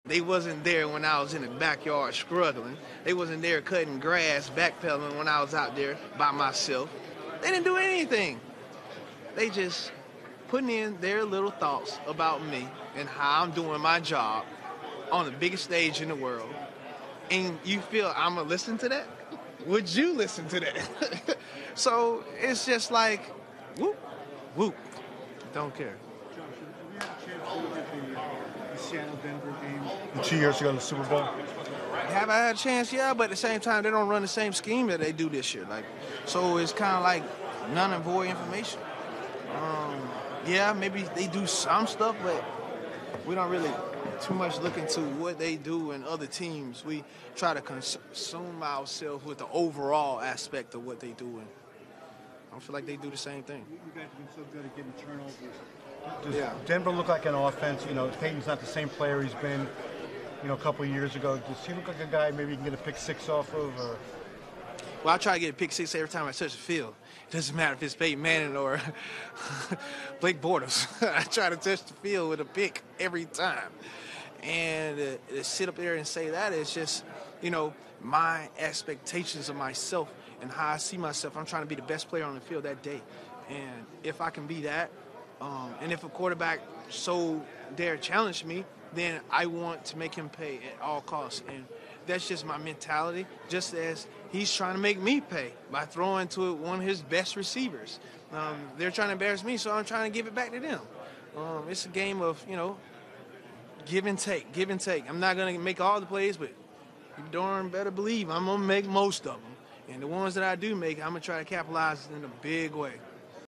Josh Norman on Facing Peyton Manning, "I Try to Get a Pick Six Every Time I Touch the Field"
Carolina Panthers cornerback Josh Norman discusses his desire to get an interception on Denver Broncos quarterback Peyton Manning.